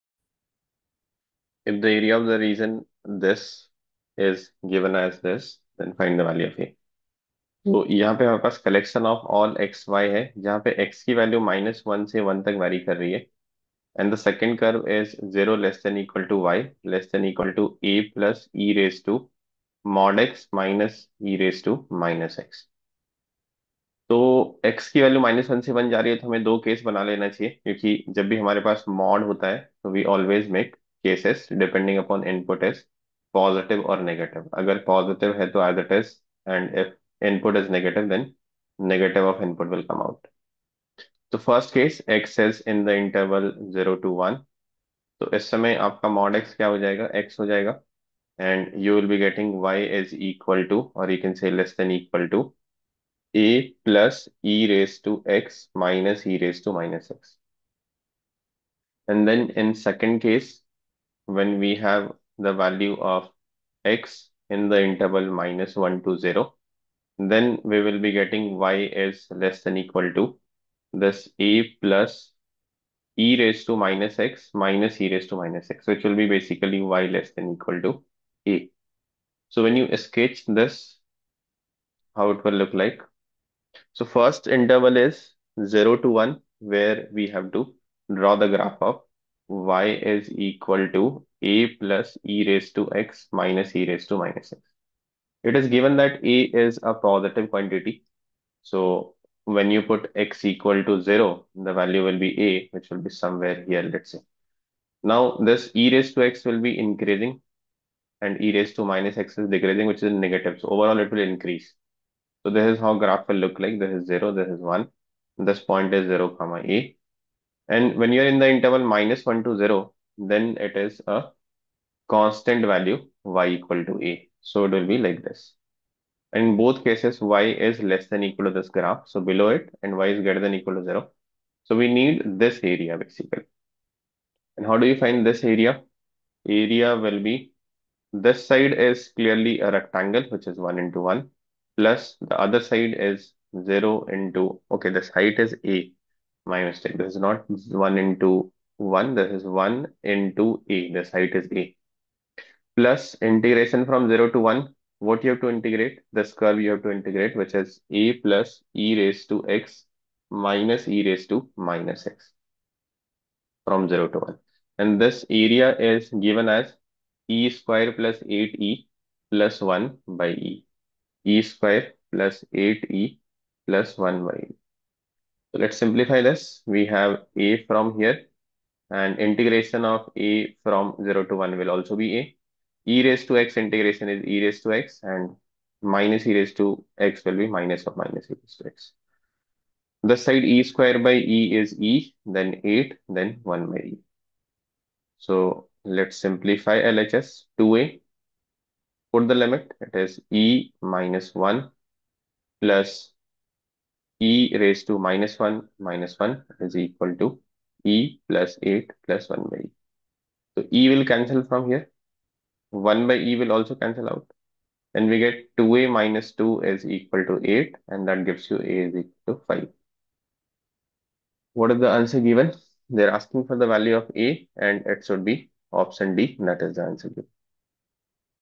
If the area of the region {(x,y) : -1 ≤x≤1 , 0≤y≤a+e^| -e^(-x) ,a 0 } is (e^2+8e+1)/e , then the
23rd Jan Shift 2, JEE MAIN 2025 Area
If the area of the region {(x,y) : -1 ≤x≤1 , 0≤y≤a+e^|x| -e^(-x)  ,a 0 } is (e^2+8e+1)/e , then the value of a is :
A) 7  B) 6  C) 8  D) 5  [ Answer – D]